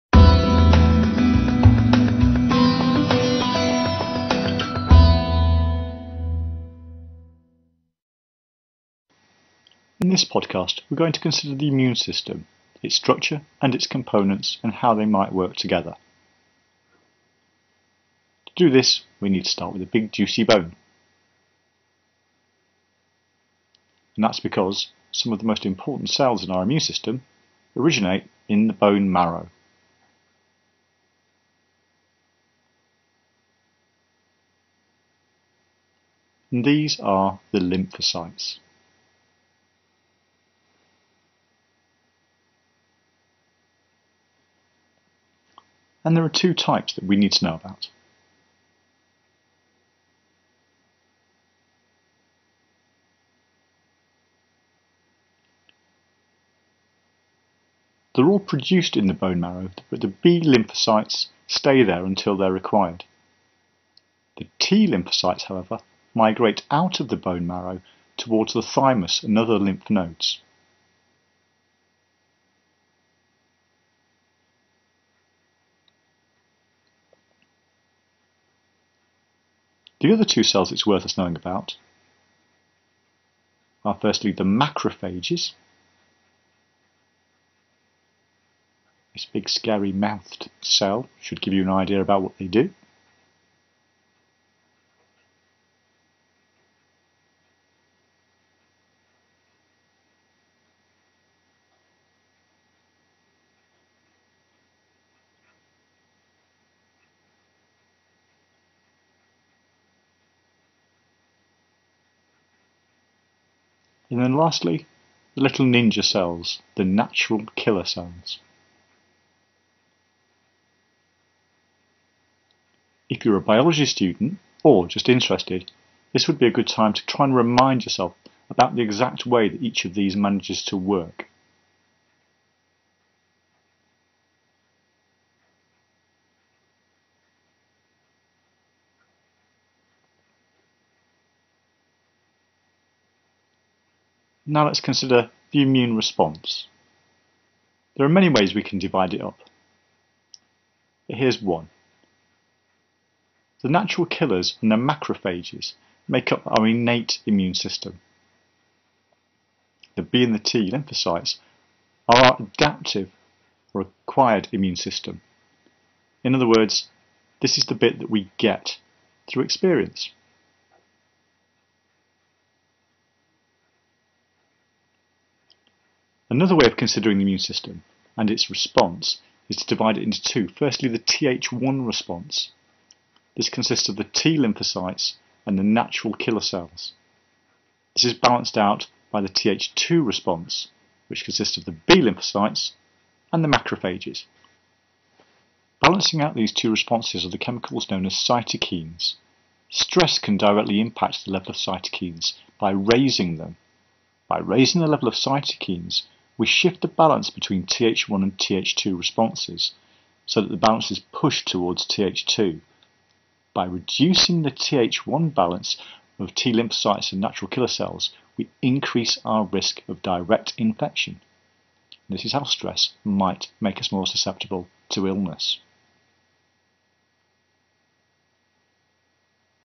Immune System
A short video looking at components and divisions of function within the immune system.